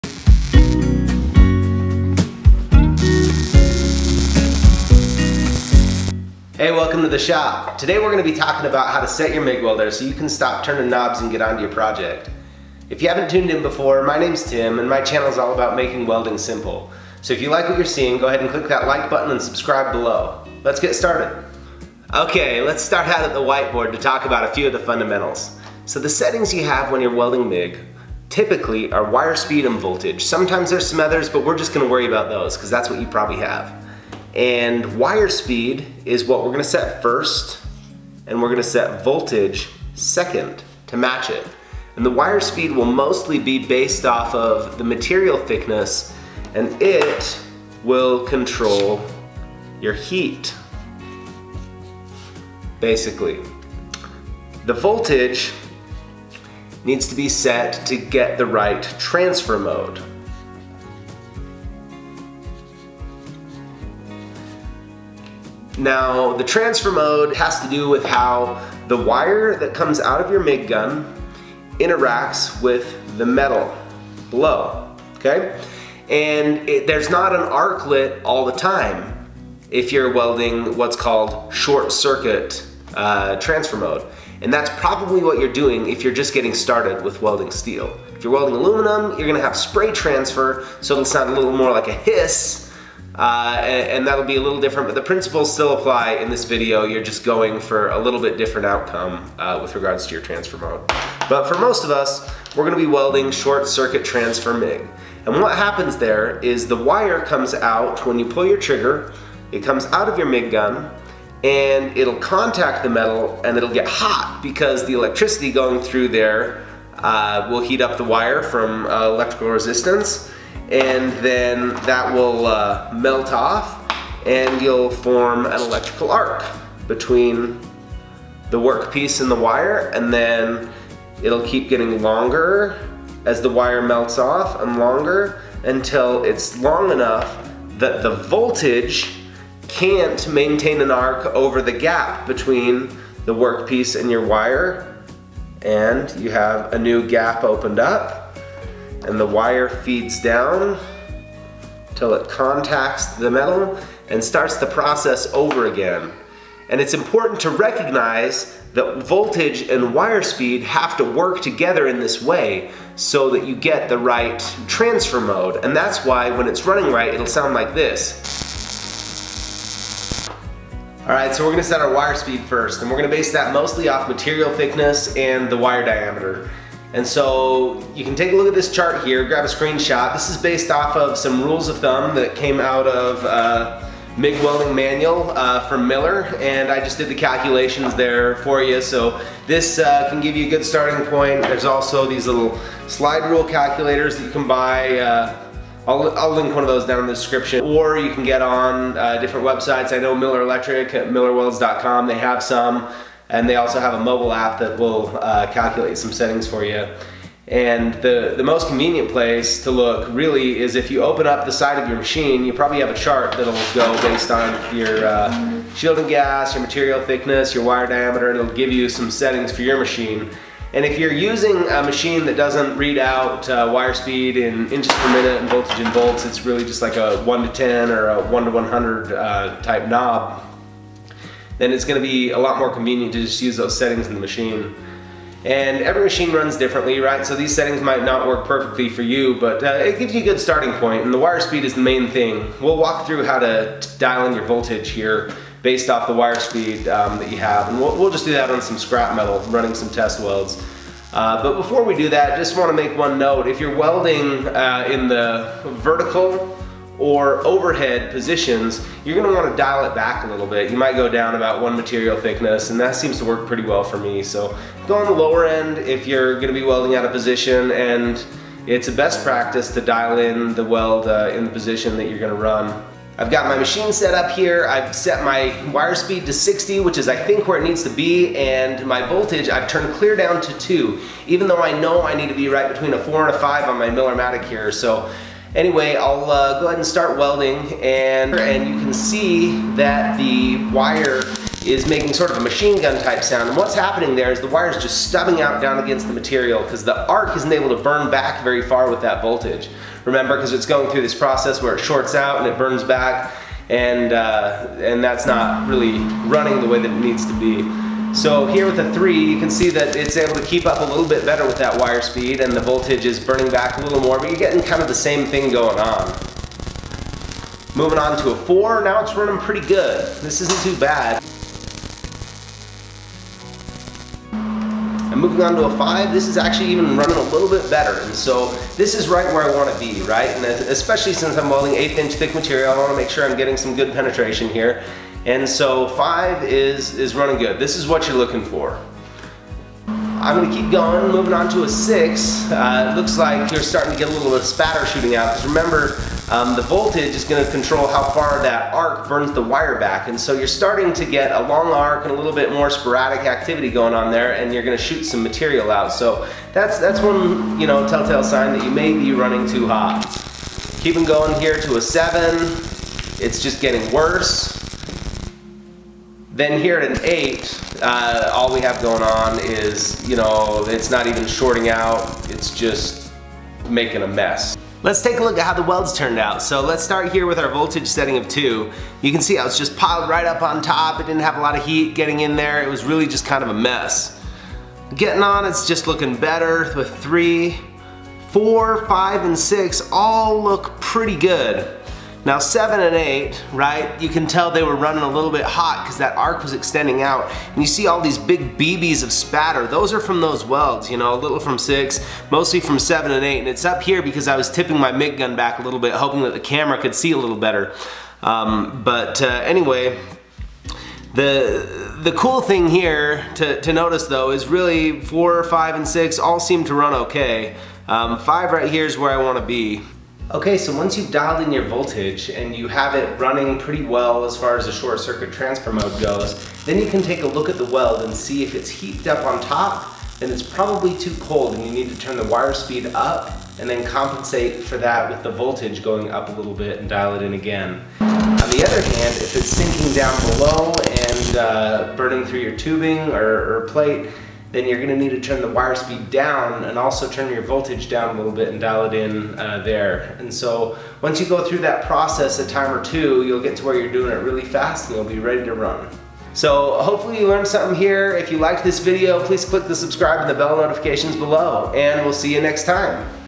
MIG Welder Settings: MIG Welding Basics for Beginners
How to set wire feed speed and voltage on a MIG welder.

The wire speed on a MIG welder will be based on material thickness. The wire feed speed and voltage for 1/8 inch thick steel will be lower than for 1/4 inch thick steel. In this video I will walk you through the process of adjusting your MIG welding settings to get a quality weld. This is part of a series on how to MIG weld for beginners.